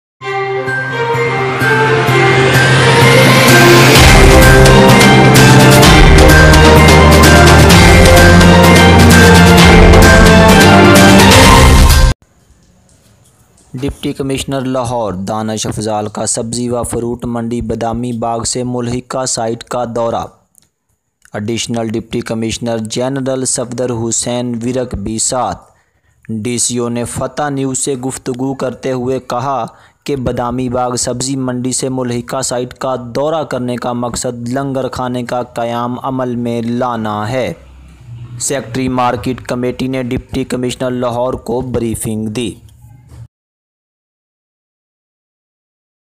ڈپٹی کمشنر لاہور دانش افضال کا سبزہ منڈی و فروٹ منڈی سے ملحقہ علاقے کا دورہ
رپورٹ تحسین احسان ڈپٹی سی ای او فتح نیوز